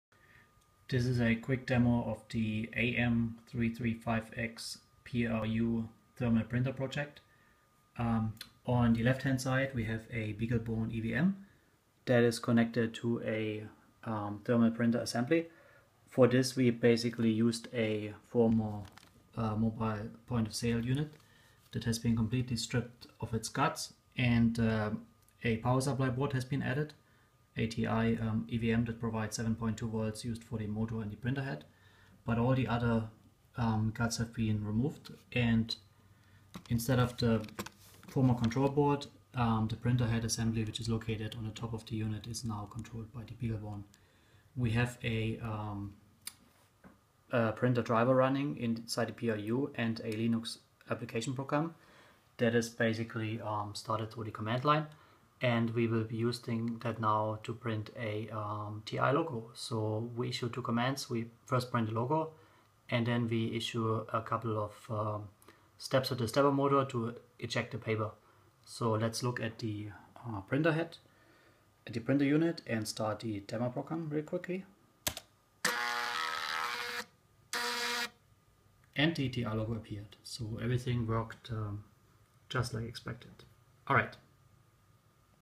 TI AM3359 PRU-Based Thermal Printer Project
Small project that uses the TI AM3359 SoC's built-in PRU (Programmable Real-time Unit) to interface with an external thermal printer assembly. The PRU operates completely independent of the AM3359 main Cortex-A8 CPU and can generate all of the needed us-precise timings for controlling the printer assembly without any real-time concerns that normally affect embedded Linux programs. The PRU firmware processes "print jobs" which are stored in a shared memory region by the AM3359's main CPU. A simple Linux command line application was written that takes a PNG image, converts it into a "print job", and kicks off the printing process which will be executed entirely by the PRU.